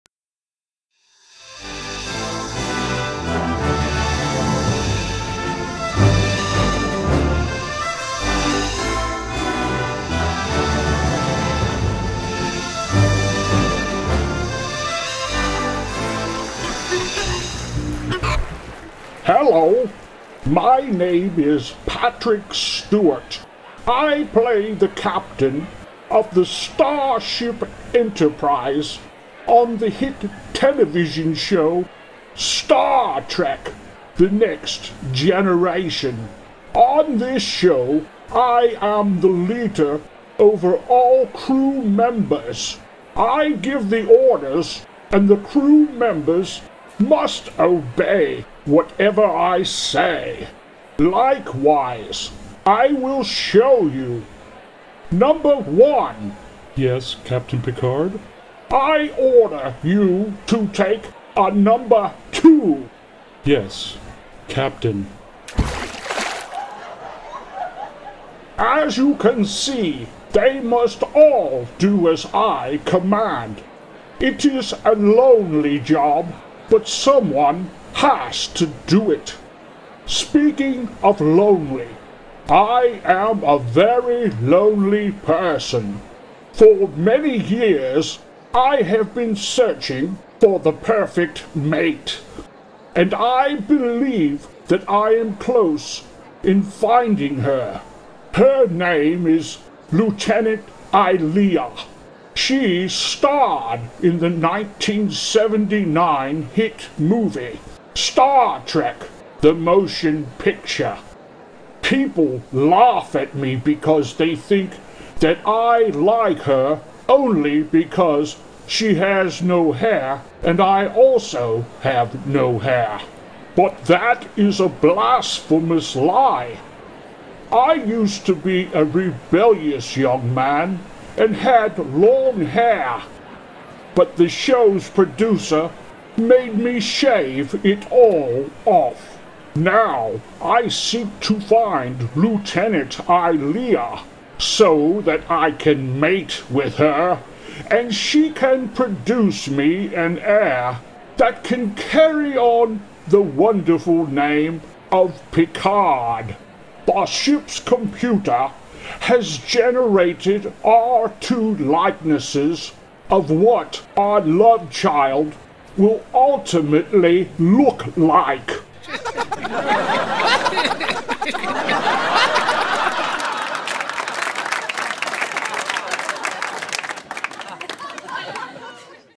Star Trek: Picard Second Season
After the disastrous 1st season of Star Trek: Picard, the producers thought that the show should change it's format more toward the the old days of TOS and add good humor to entertain all of the fans of Star Trek.